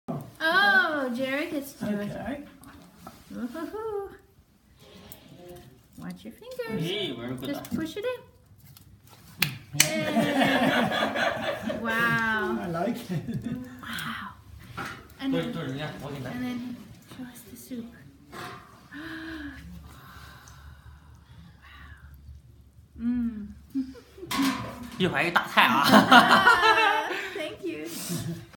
Goose anyone?
Fresh goose stew and vegetables cooked in a very traditional Chinese pot.